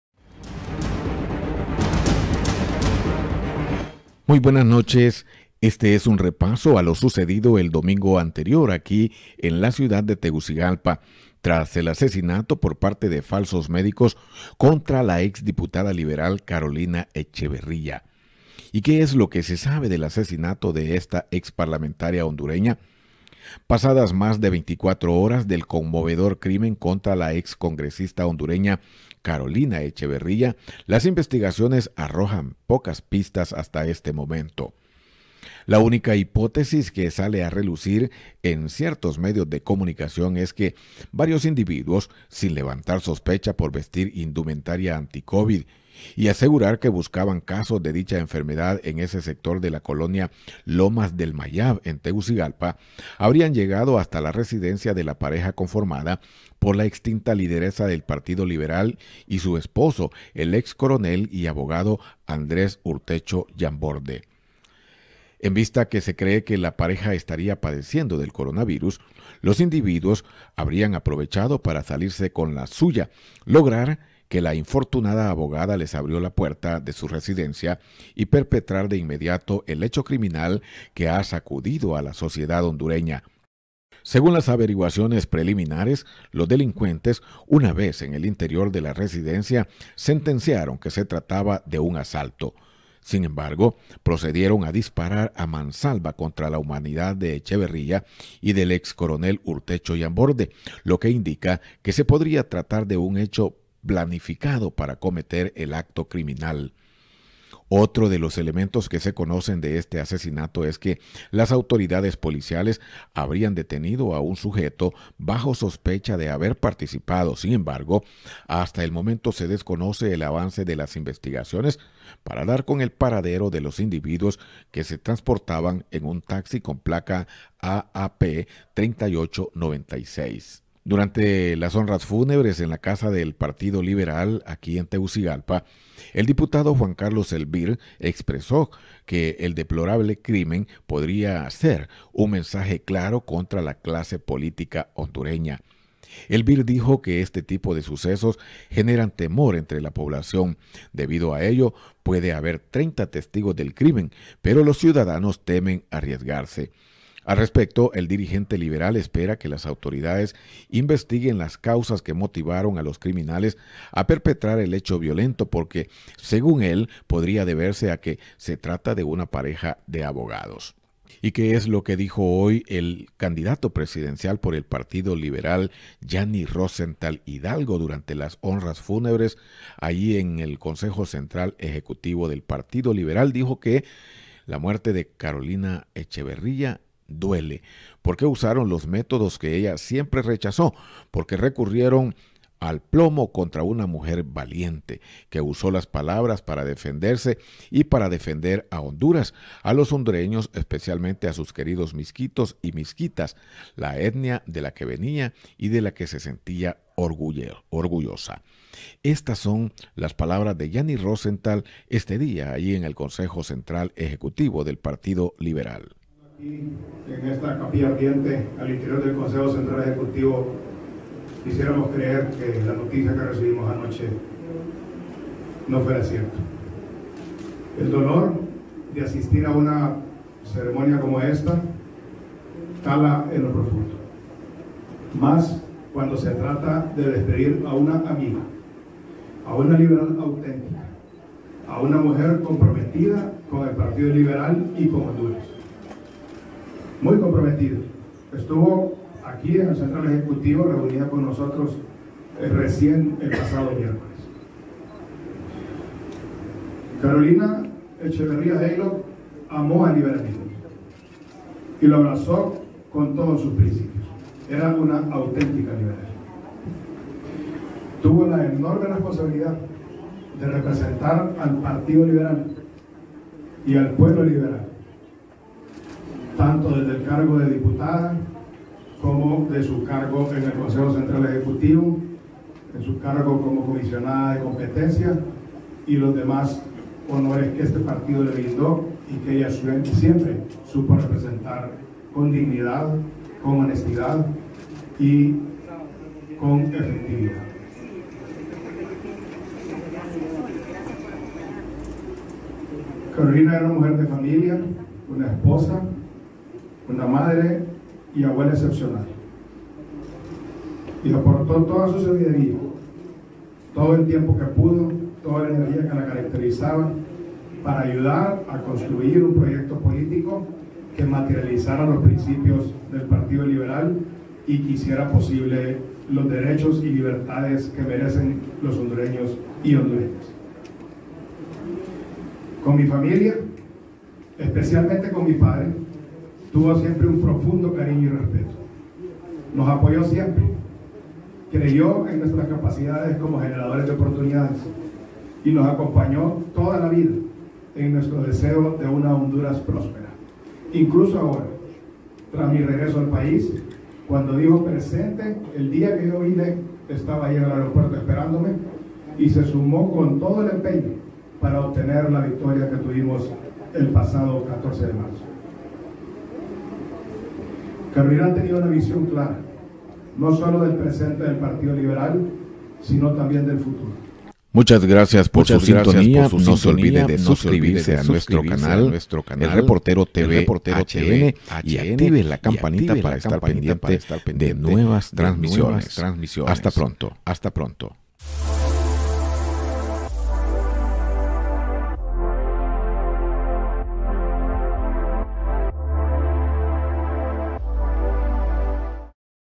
Lo que se sabe del asesinato de la ex parlamentaria hondureña, Carolina Echeverría
Pasadas mas de 24 horas del conmovedor crimen contra la ex congresista hondureña, Carolina Echeverria las investigaciones arrojan pocas pistas.